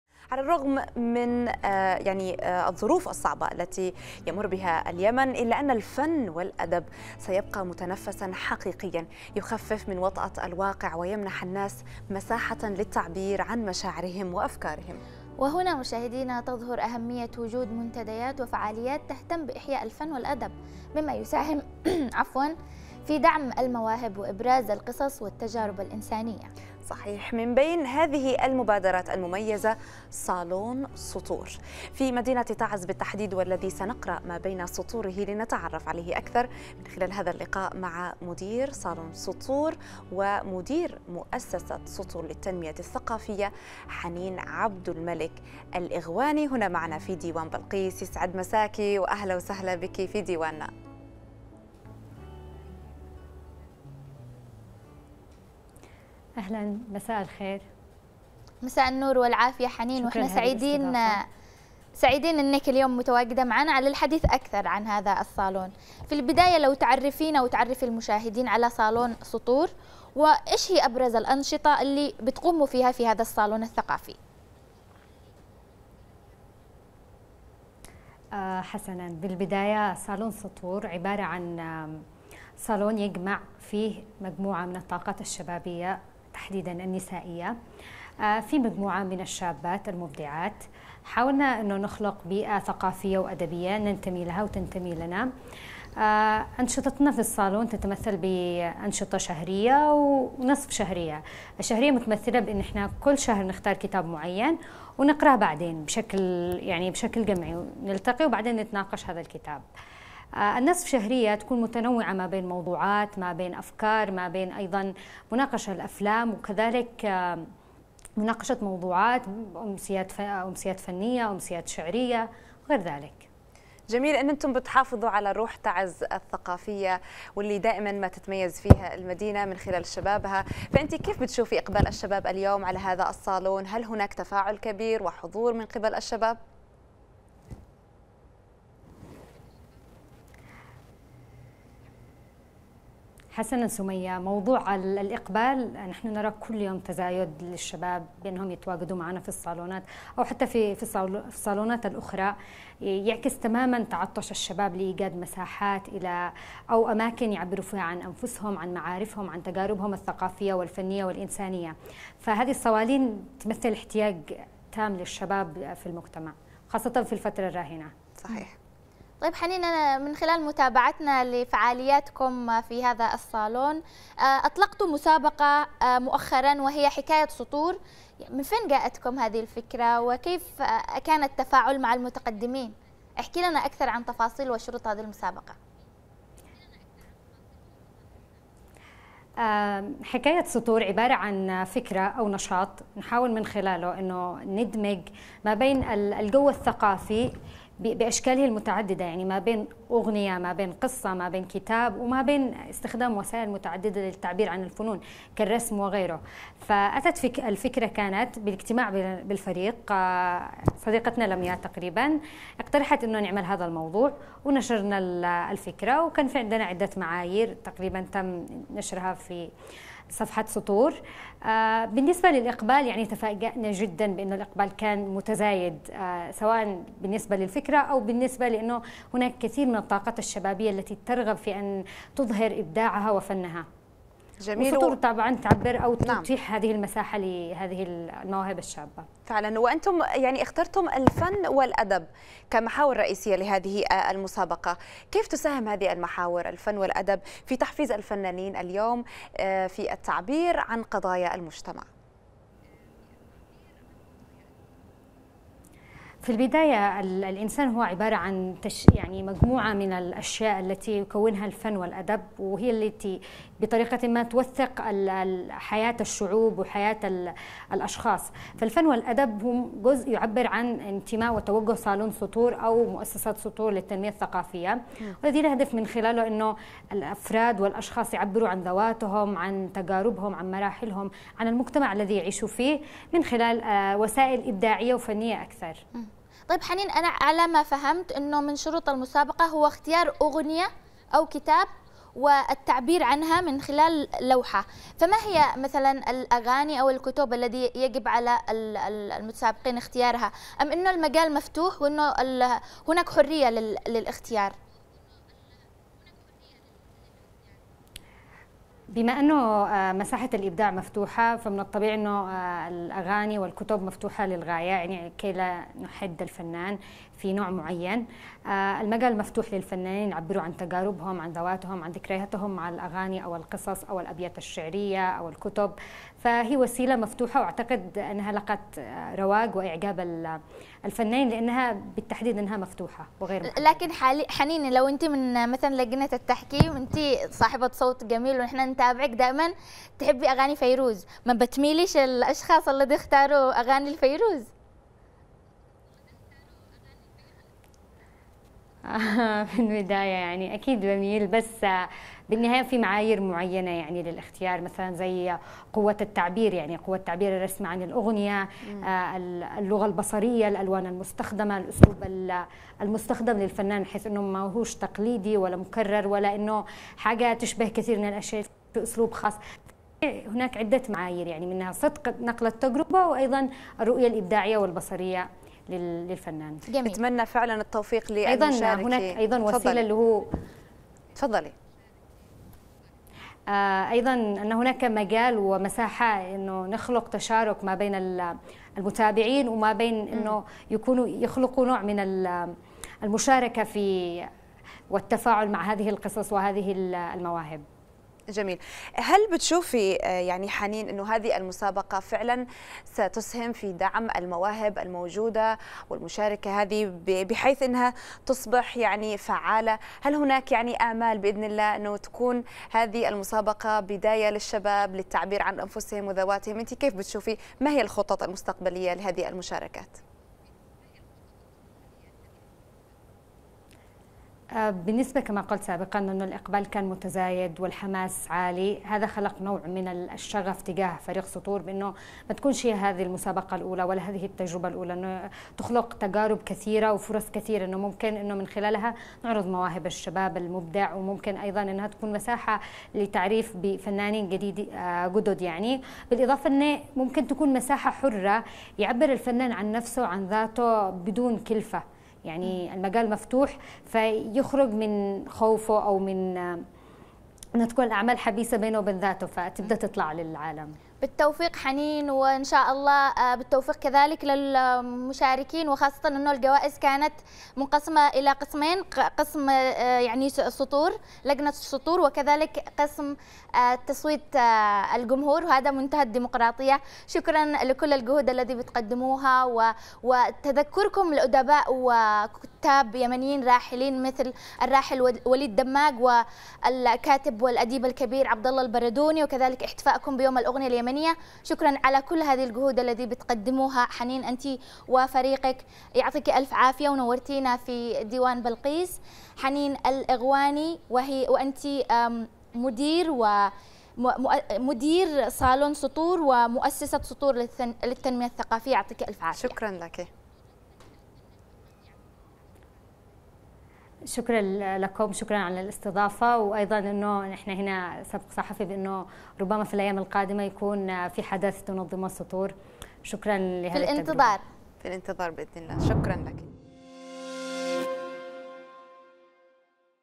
ديوان بلقيس | سطور.. صالون ثقافي أدبي في تعز وسط عتمة المشهد - مع حنين الإغواني
يبقى الفن والأدب  متنفسًا حقيقيًا يخفف من وطأة الواقع ويمنح الناس مساحة للتعبير عن مشاعرهم وأفكارهم..
وهنا تظهر أهمية وجود منتديات وفعاليات تهتم بإحياء الفن والأدب، مما يساهم في دعم المواهب وإبراز القصص والتجارب الإنسانية .
من بين هذه المبادرات المميزة "صالون سطور" في مدينة تعز والذي سنقرأ ما بين سطوره لنتعرف عليه أكثر في لقائنا التالي مع مدير صالون سطور ومدير مؤسسة سطور للتنمية الثقافية حنين عبد الملك الإغواني